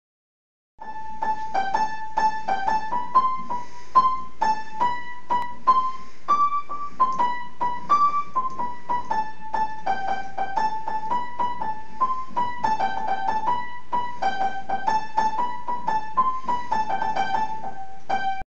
Thomas forte
We have been gifted with a loan of a baby grand, Thomas tries it out.